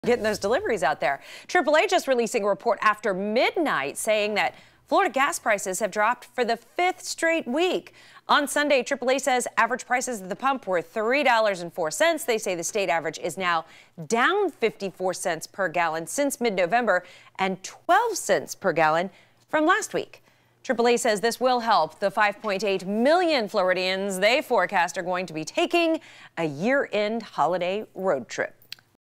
Florida gas prices drop for 5th straight week, AAA reports
The drop in prices will help the more than 5 million Floridians expected to travel by car during the holidays.